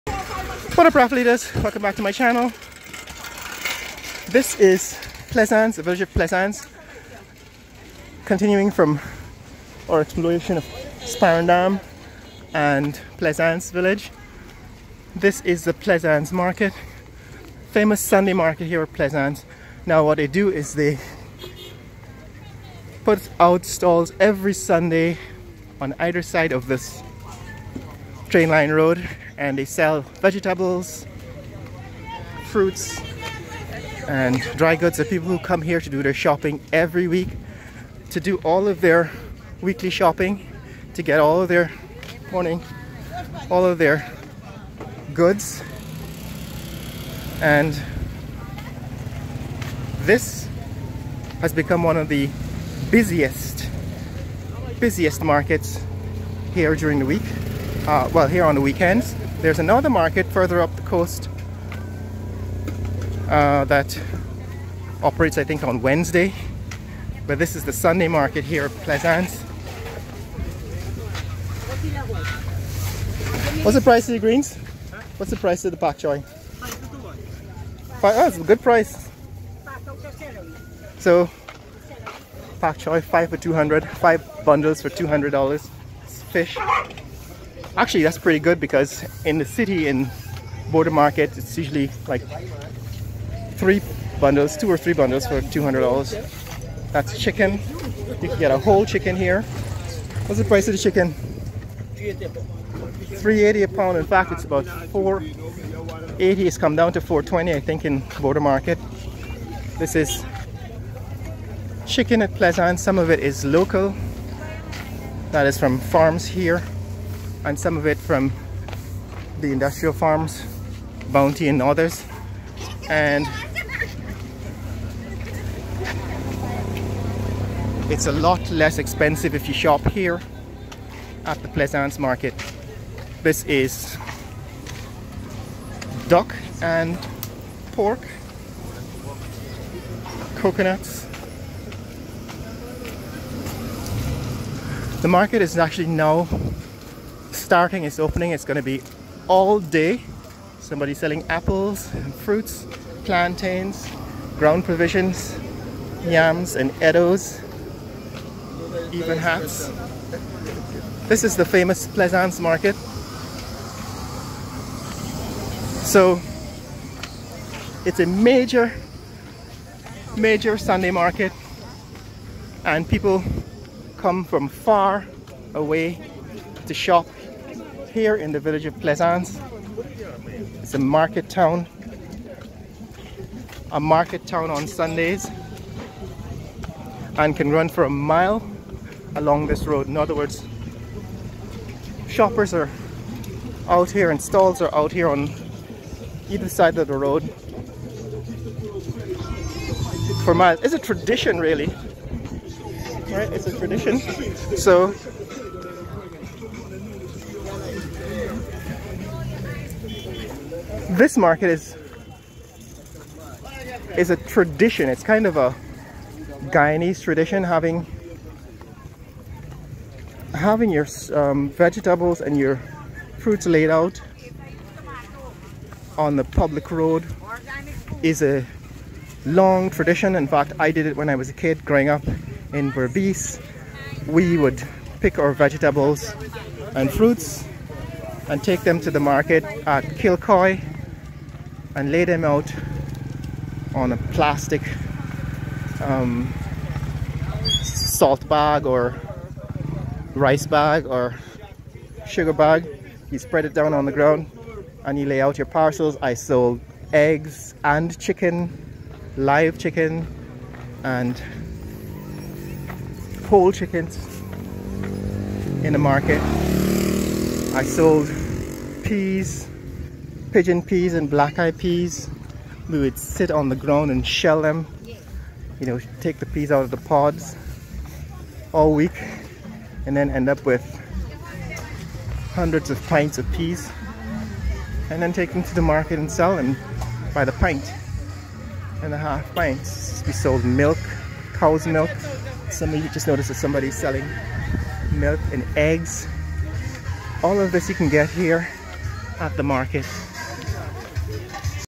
Plaisance Sunday Market - Farmers Market - Part 1
Walk with me along the train line road to see the famous Sunday Market at Plaisance. This is a farmers market that is a major destination for shoppers every Sunday as farmers being their produce to sell directly. The prices are usually cheaper than at the major markets in the city, Bourda and Stabroek and the produce is always fresh unlike at other markets.